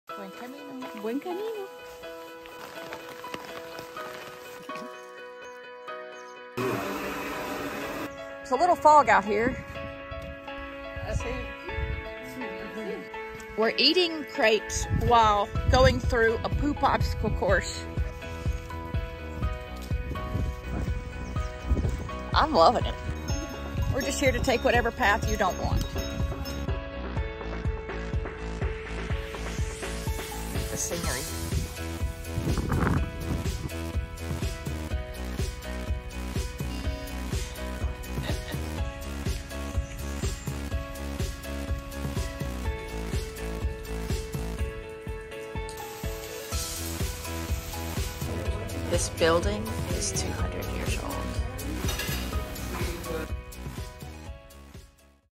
Camino de Santiago - Frances Route Day 26 - Shorts from the Trail
Mom and daughter walk from O Cebreiro to Triacastela along the Camino de Santiago.